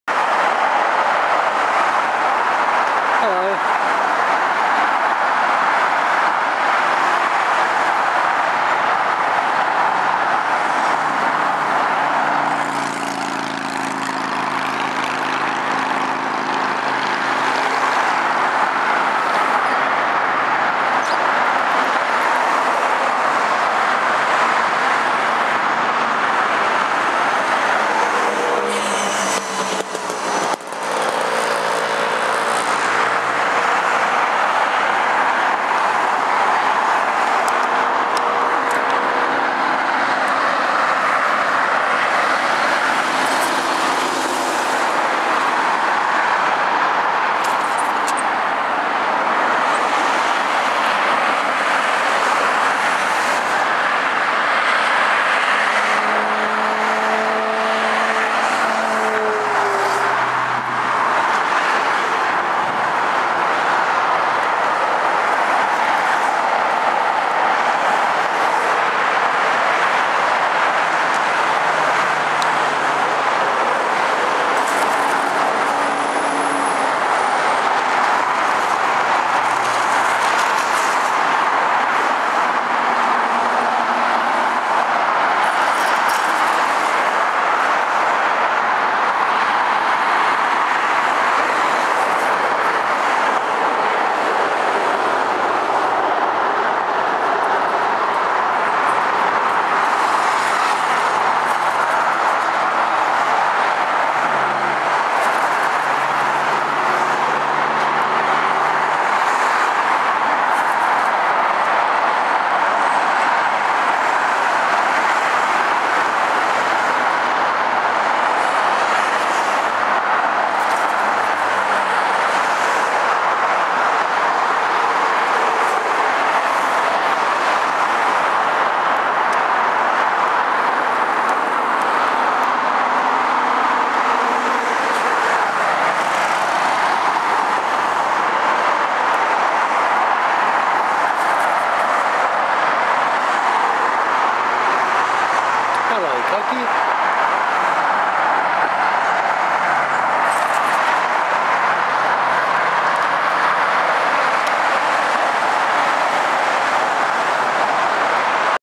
Brave Cockies
This is probably the same group of cockies I filmed nine months ago in the video "Cockies in Melbourne's Inner East". They're completely wild of course but in that particular environment they tolerate me coming within inches of them.

Filmed in Suburban Melbourne, Australia.